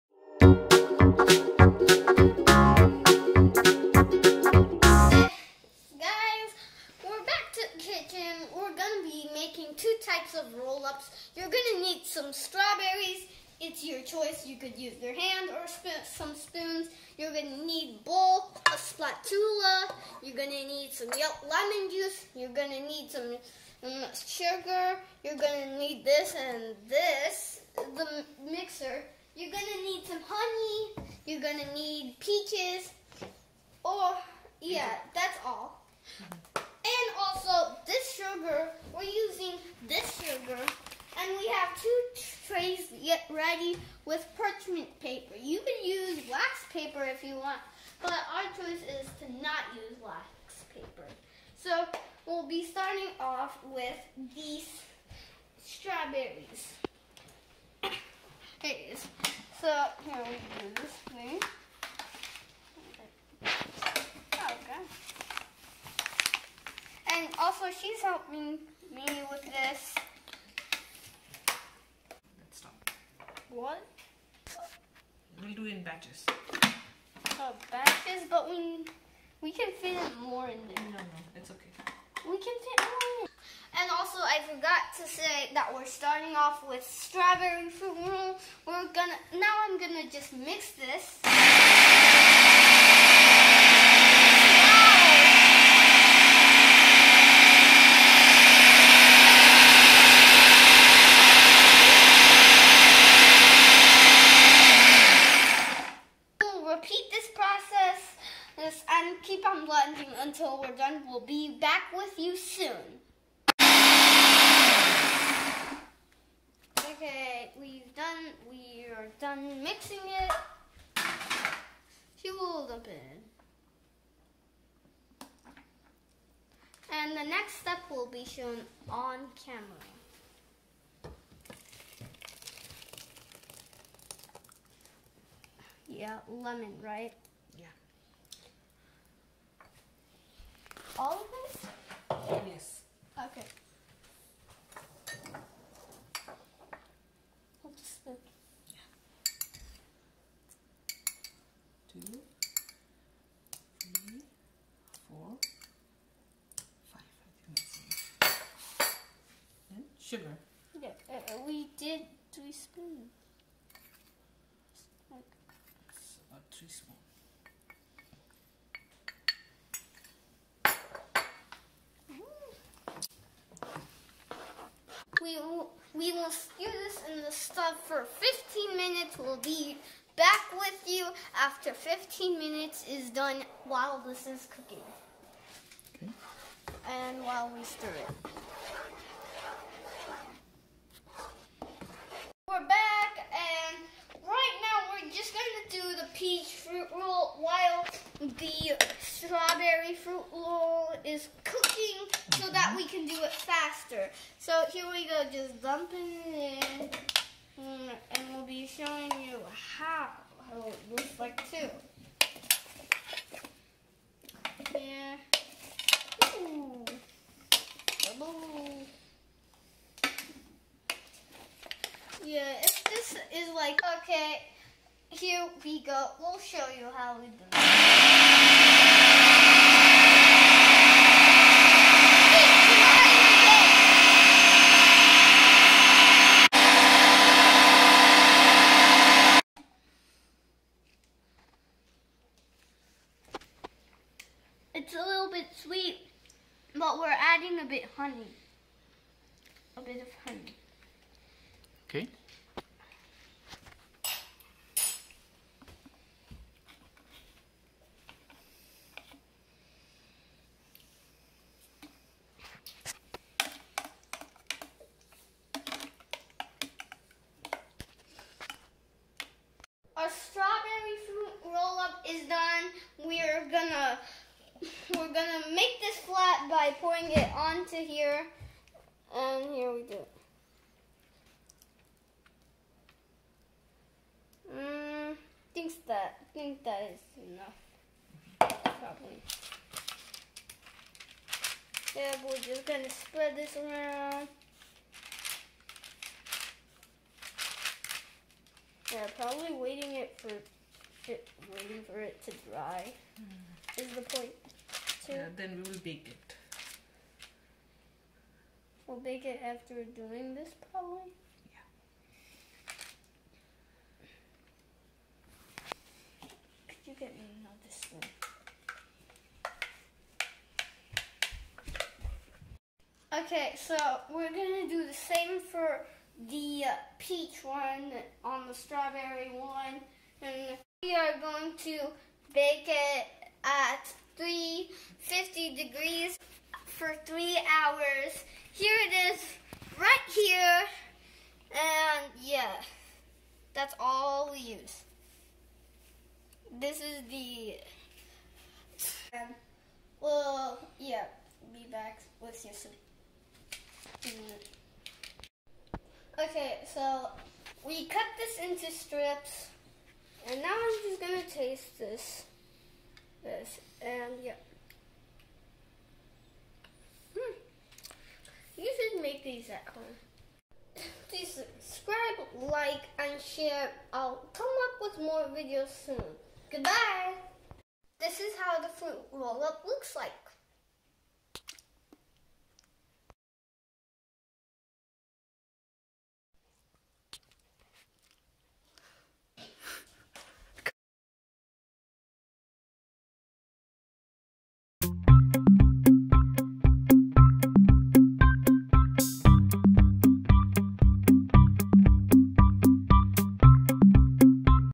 Aryan's Fruit roll up
One of Aryan's summer activities was to make fruit roll-ups. With the abundance of strawberries and peaches, he decided to use easy blending, sweetening and dehydrating process to make sweet and sour, home-made fruit rollups. Enjoy his recipes